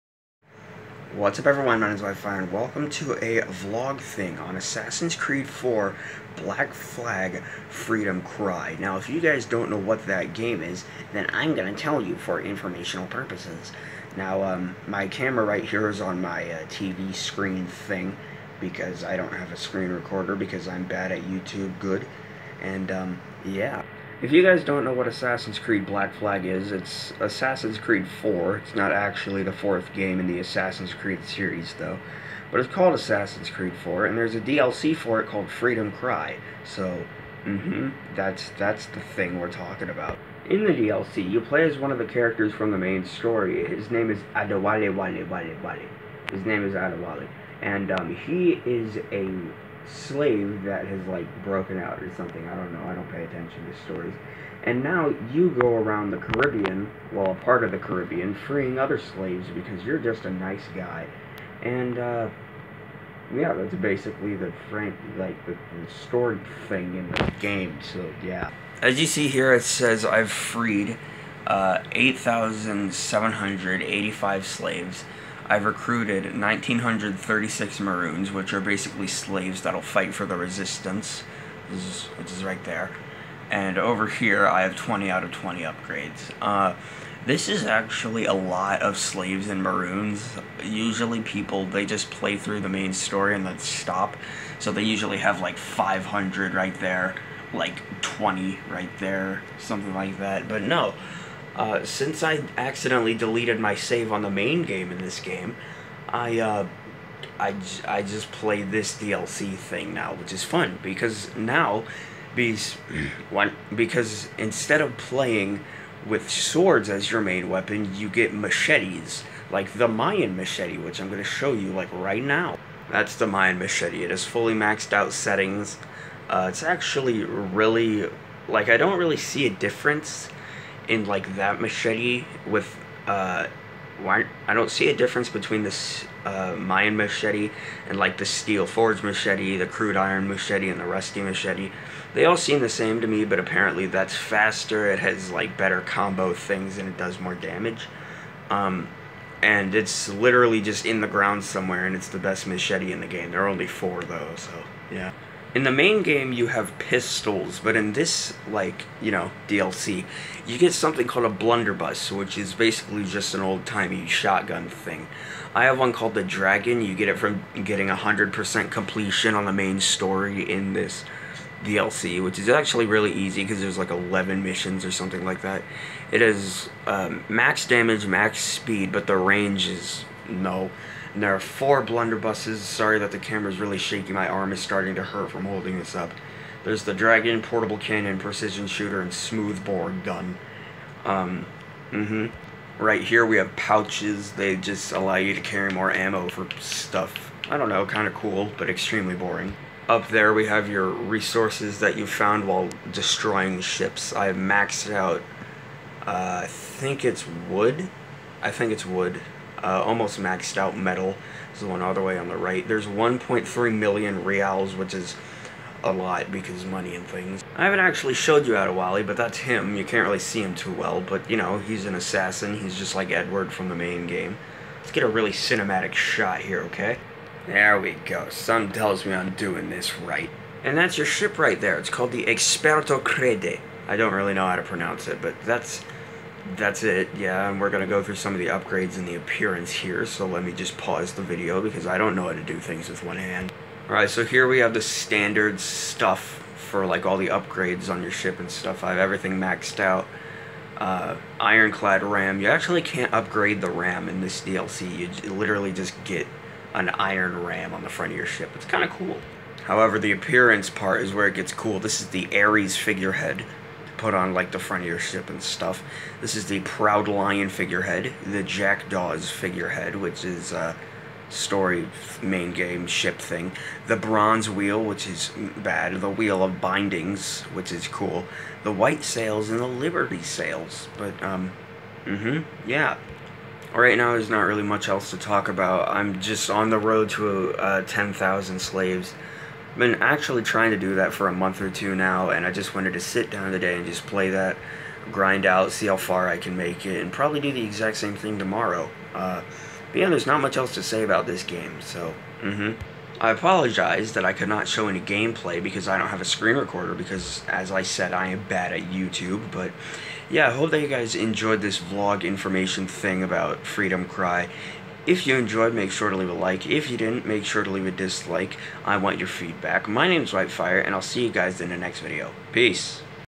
Assassin's Creed 4: Black Flag: Freedom Cry Info
Just some information about a game that I love but can not show to you guys in full effect. I hope you learned something about this game in this video, thank you for all of the support on all of my videos... so yeah... bye.